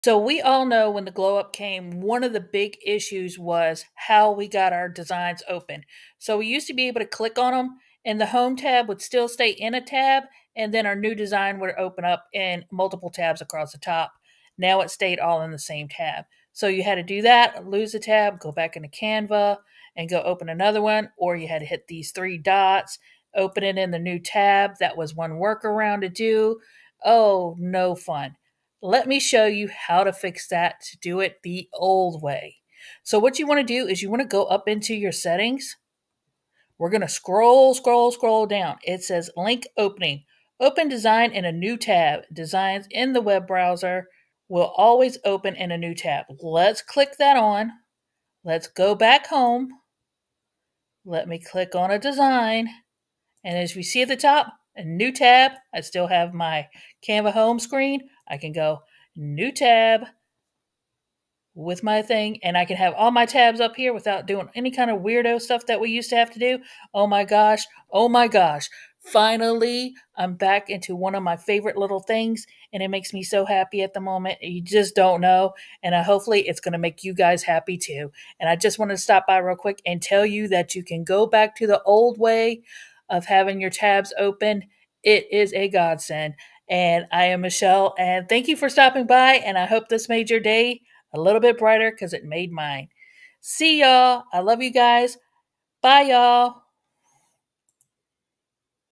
Multiple Tabs the Old Way and How to Do It in Canva
Some of the Canva Glow up was a glow down and Canva listened! Now you can do multiple tabs the old way and I will show you how